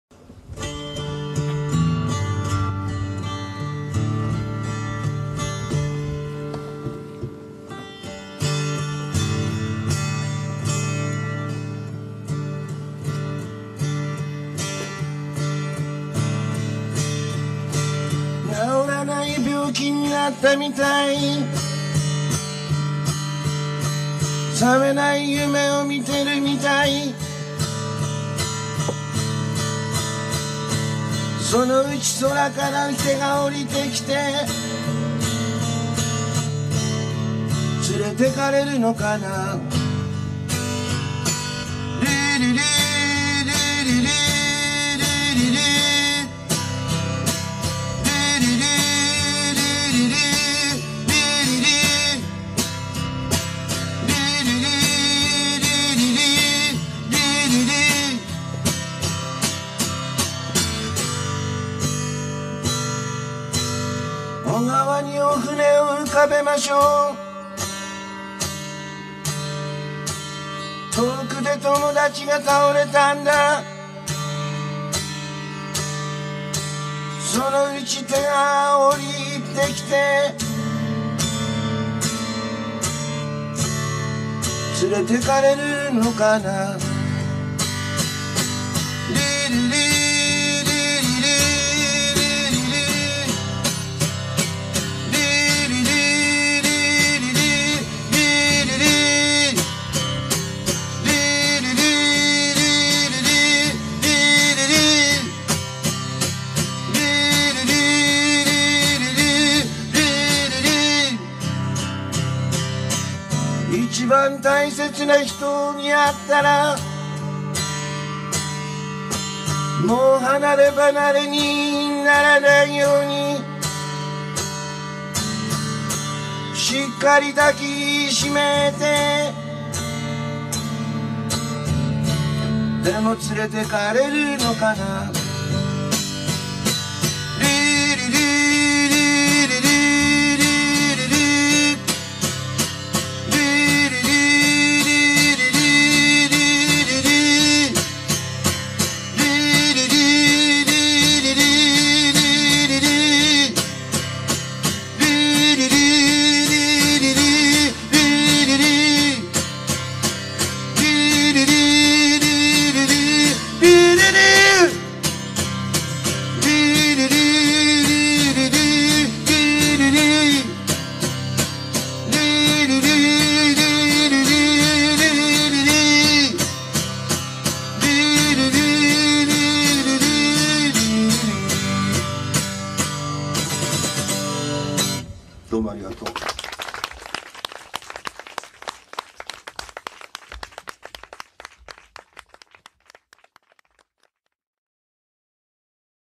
宮沢正一 渋谷アピア 2003.3.16
TOUCH-MEのフロント・アクトとして出演。